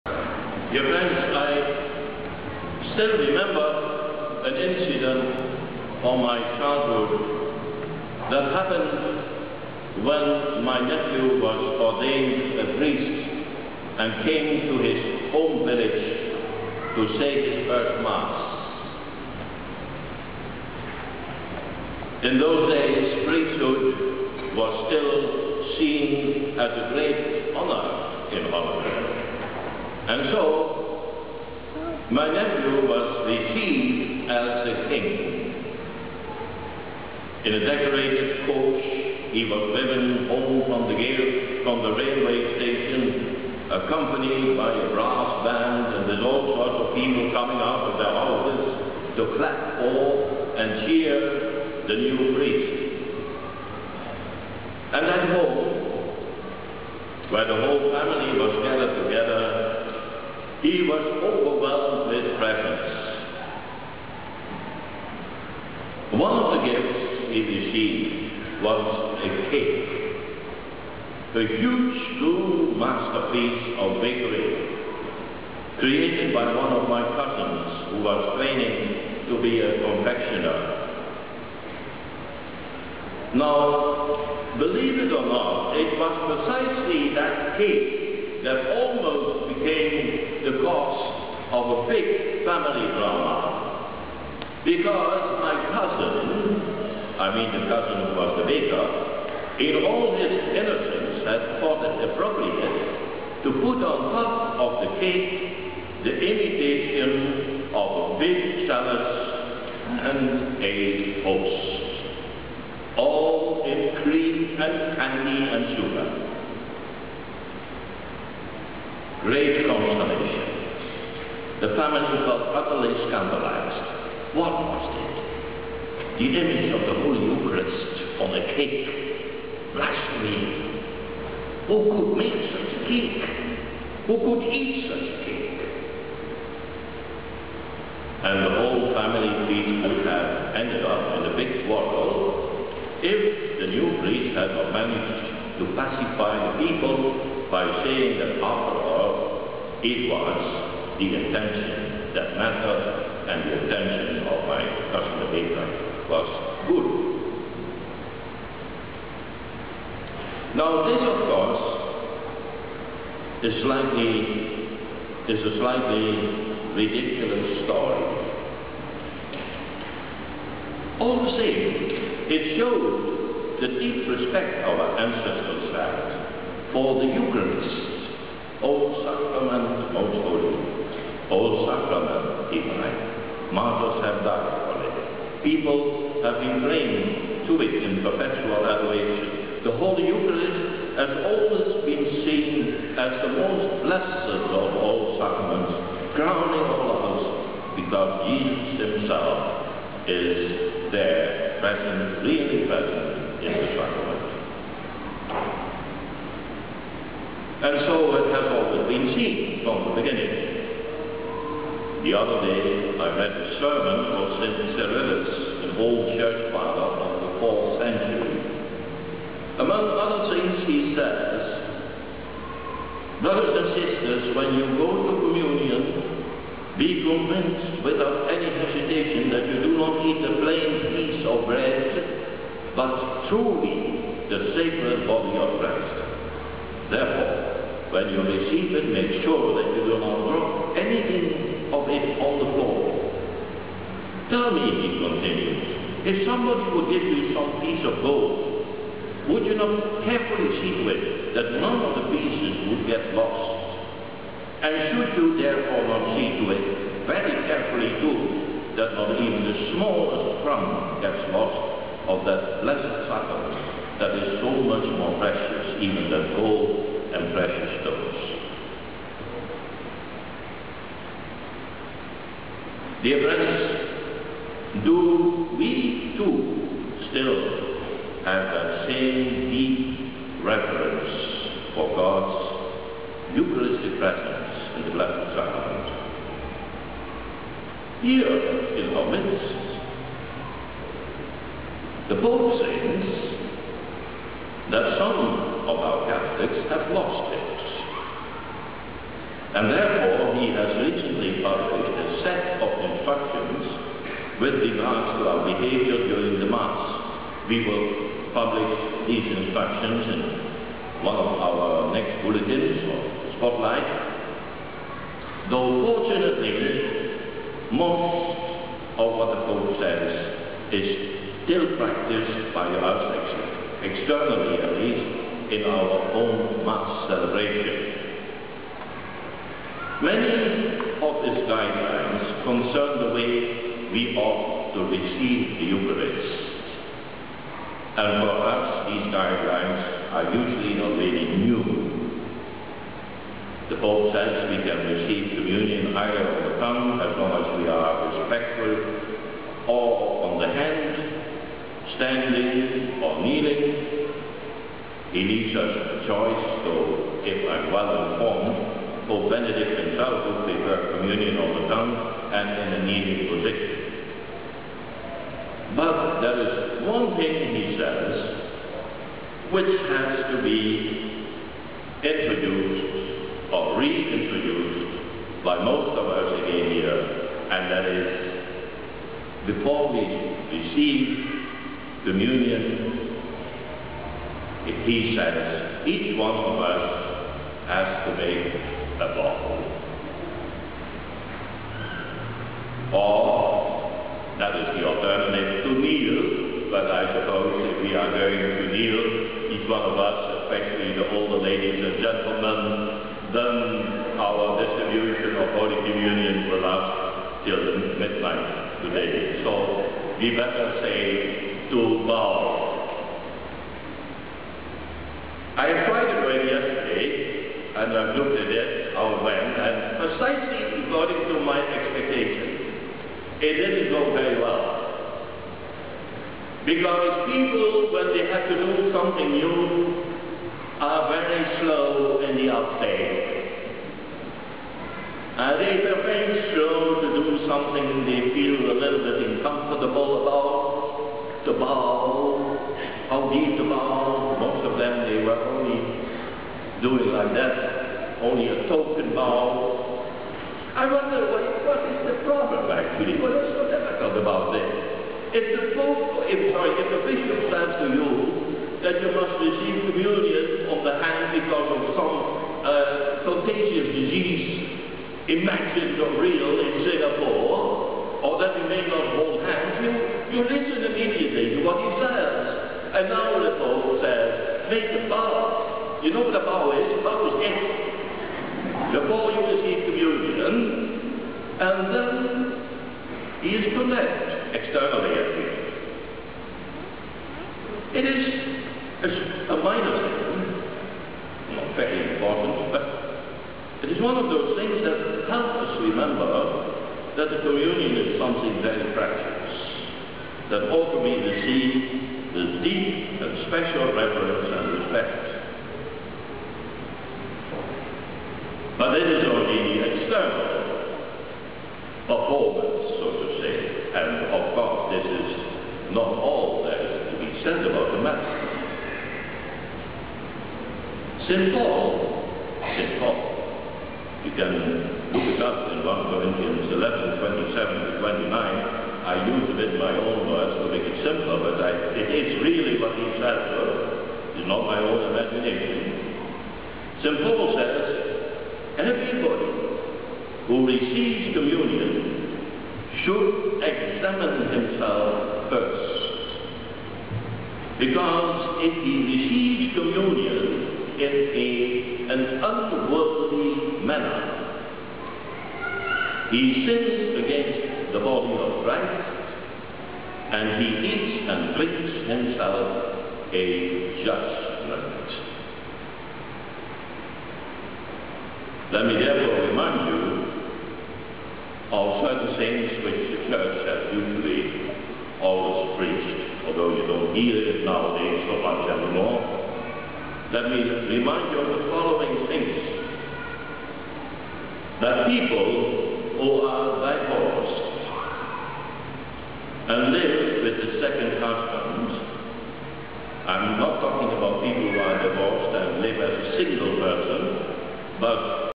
Due Respect for the Eucharist (Part 1)
Gone were the days when the Eucharist was given utmost reverence because Jesus is truly present there. Fr Anthony Hutjes urged the parishioners of Blessed Sacrament Church on 7 March 2010 to restore a due deep respect for Eucharist in order to overcome temptations and sins. This can be done by internal examination of oneself and receiving the Sacrament of Reconciliation (Confession) (where necessary) before receiving the Eucharist. He emphasized that one must let go of hatred and unforgiveness, and be transformed into the very Food we eat.